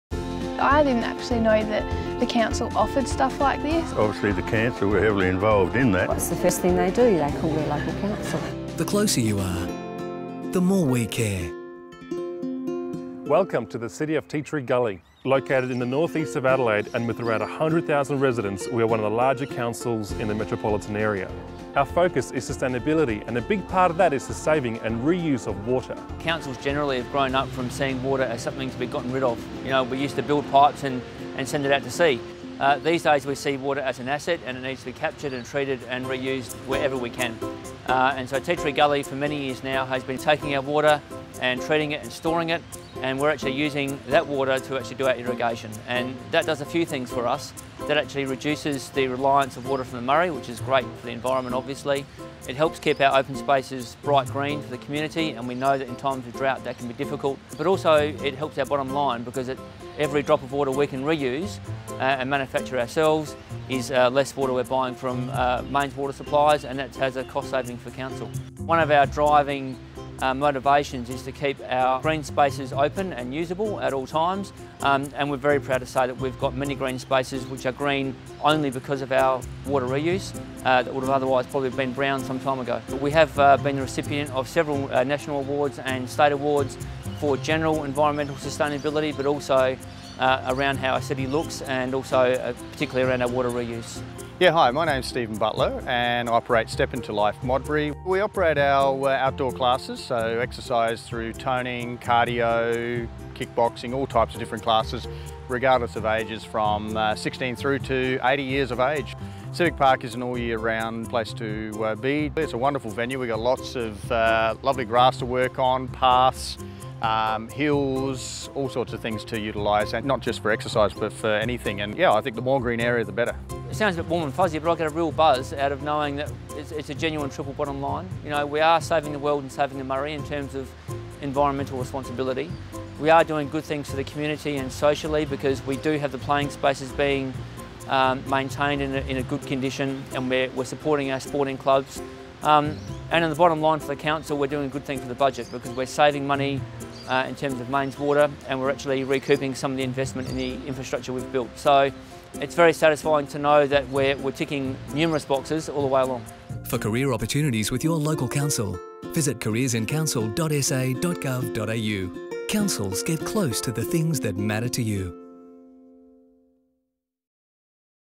Tea Tree Gully's stormwater reuse strategies
Tea Tree Gully Council staff used clever engineering solutions and attracted State and Federal funds to harvest and recycle stormwater so it can be used to water local parks and playing fields.

Council CEO Di Rogowski said Tea Tree Gully is no stranger to hatching innovative schemes to provide work for youth in the region and encouraging community engagement with Council.

"Council and our staff are very proud of the national recognition that we have received as we continue to drought-proof our city but mostly we are proud to have developed these innovative processes for our community and the environment."